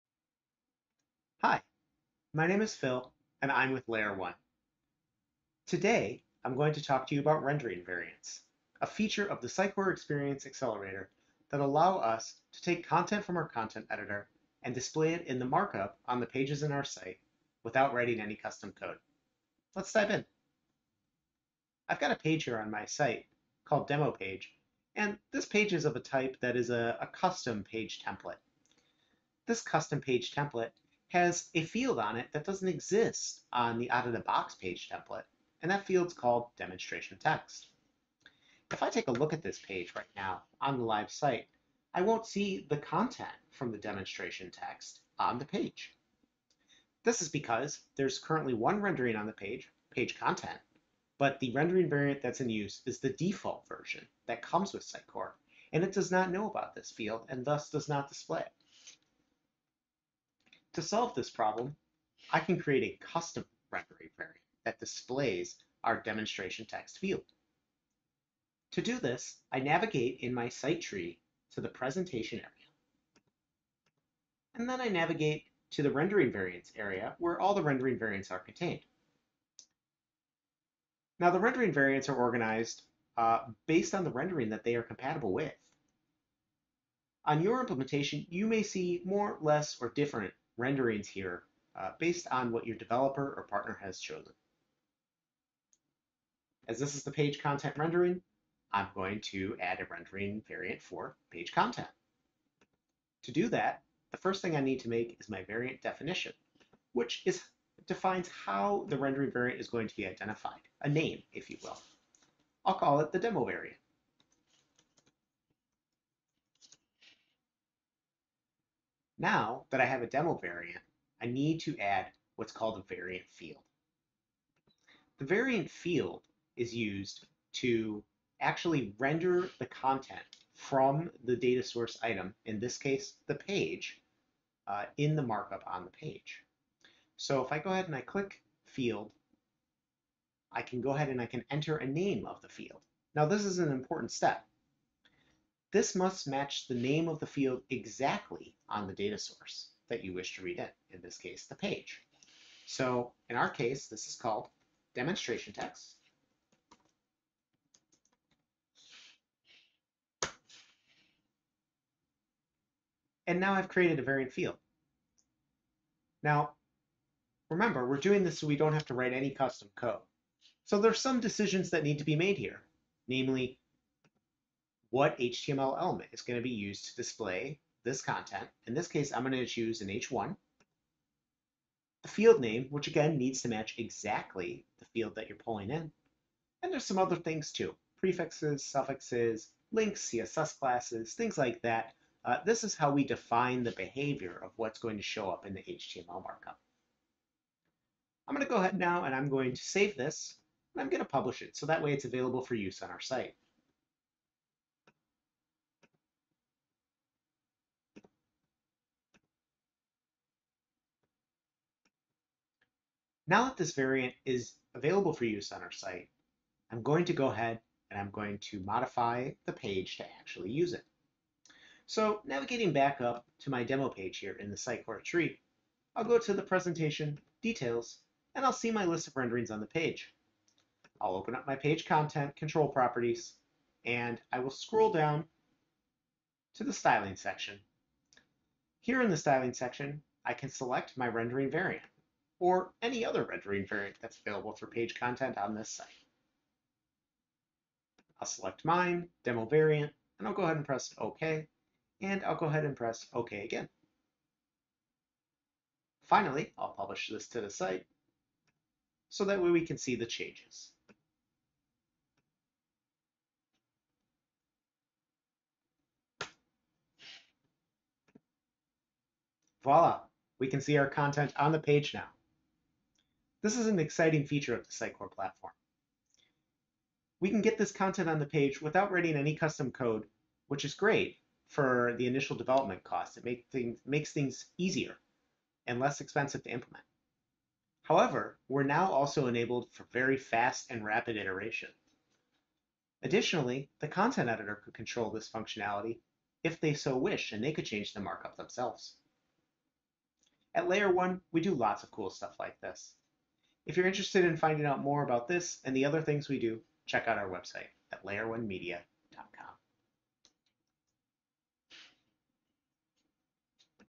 An Introduction to Rendering Variants with the Sitecore Experience Accelerator
Phil Busch, Software Delivery Team Lead, gives an introduction to Rendering Variants. Rendering Variants are a feature of the Sitecore Experience Accelerator (SXA) that allow for rapid development of markup with no or limited custom code.